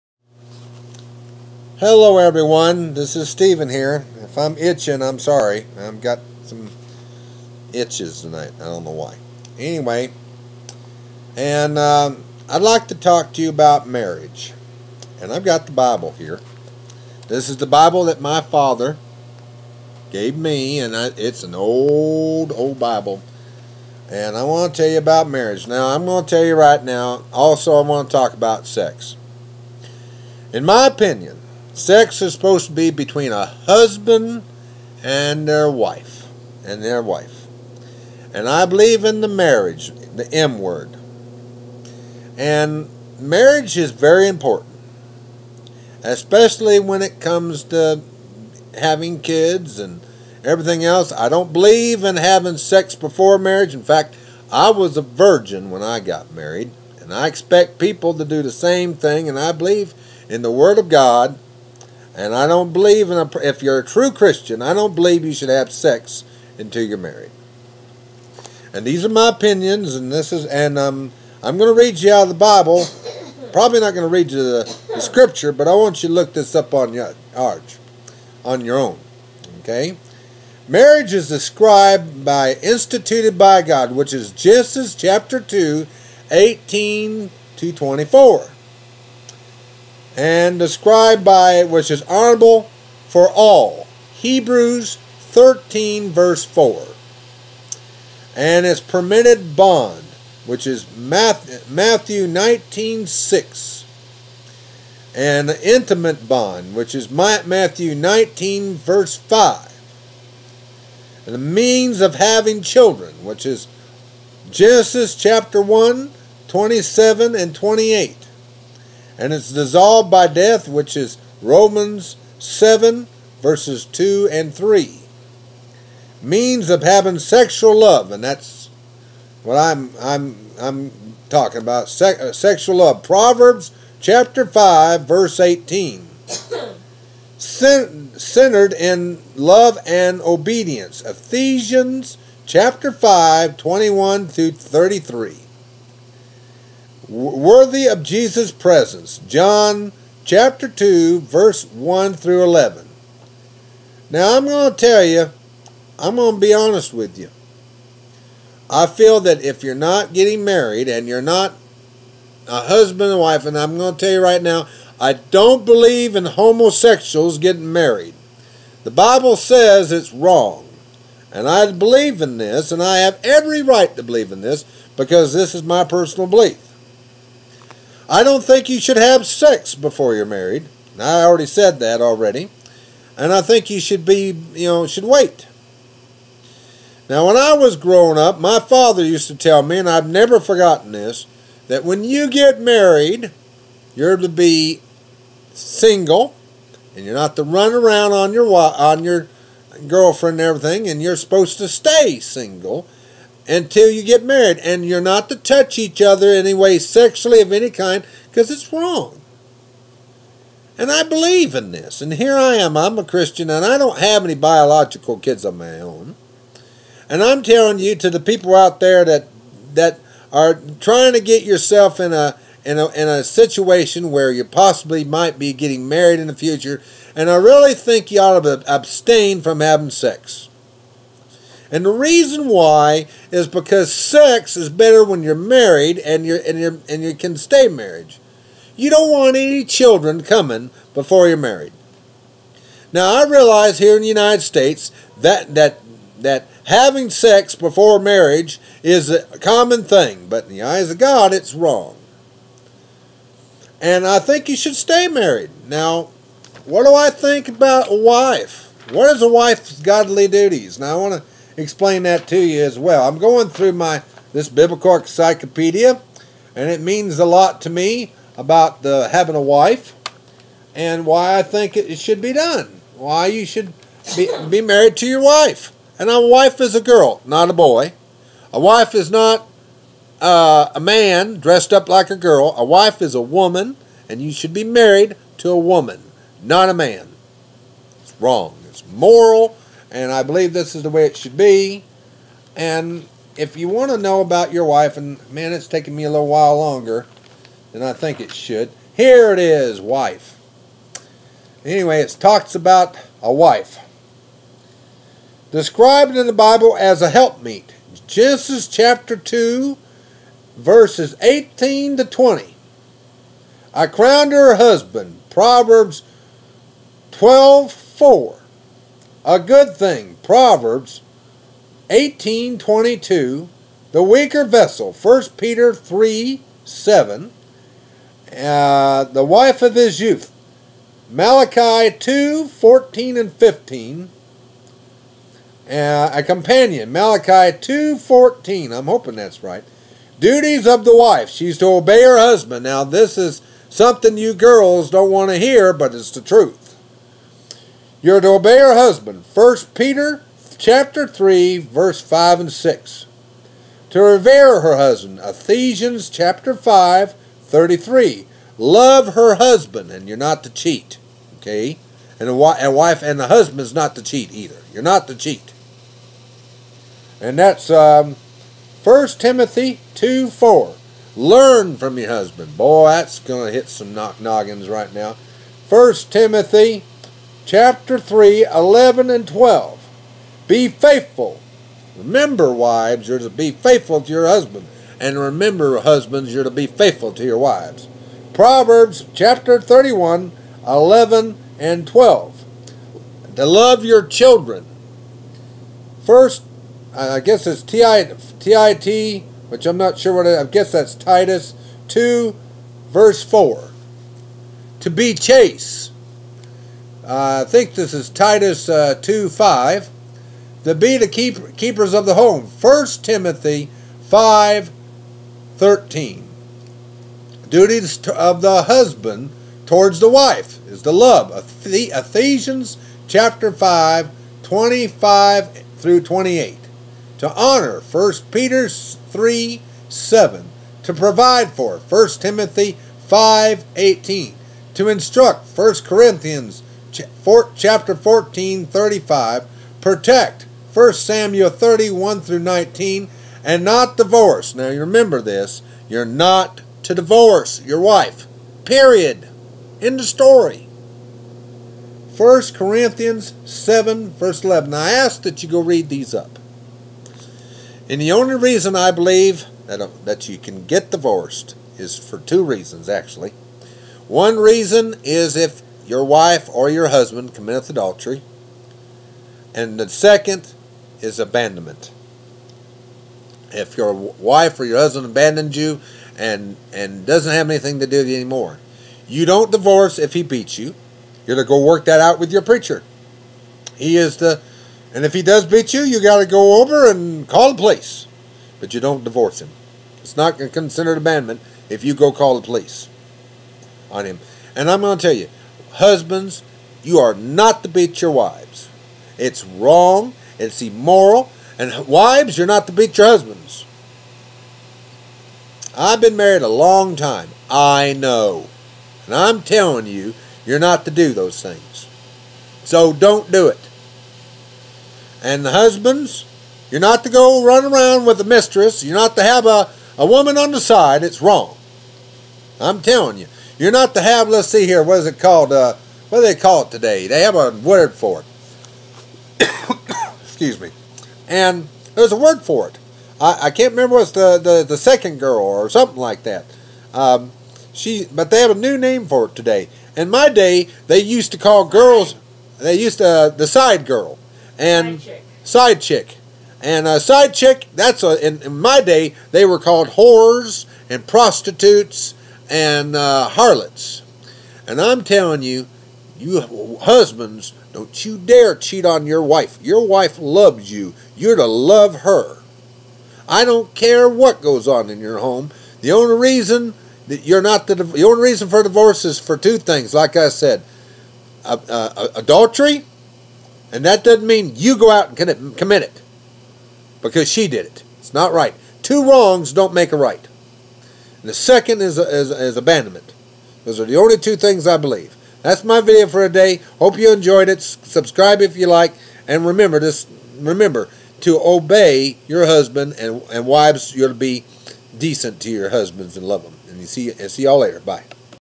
Marriage, husband and wife. Bible. No sex before marriage. being a virgin
My opinion on marriage according to the bible. and doing right.. No sex before marriage. Period. My idea of marriage between a husband and wife. Not to divorce unless you commit adultery or abandonment again this my opinion only, young couples getting ready for marriage, I don't believe in marriage of Homosexuals man being married to man or woman married to woman. Side chick is wrong in the sight of God